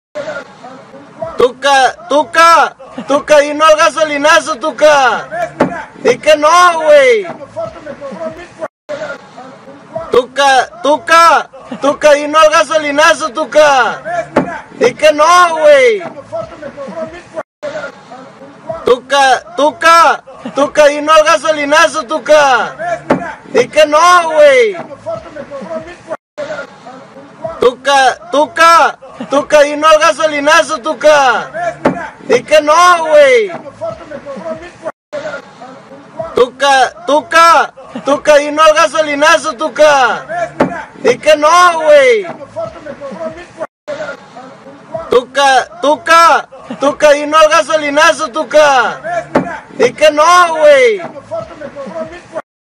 TUCA EXPLOTA CONTRA GASOLINAZO
El usuario que graba el vídeo le dice "di no al gasolinazo Tuca, di que no güey" a lo que el temperamental Tuca responde "Por un cuarto me cobró $1040".

¡SÍGUENOS!